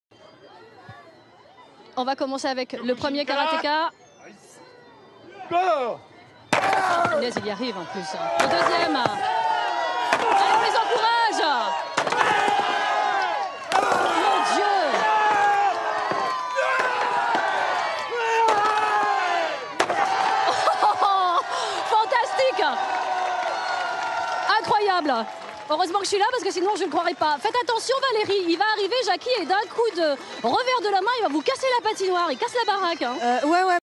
Téléthon-2012
Casse pour le Téléthon à Dreux le 10 décembre 2012 en compagnnie de Jacky Lajoinie et son dojo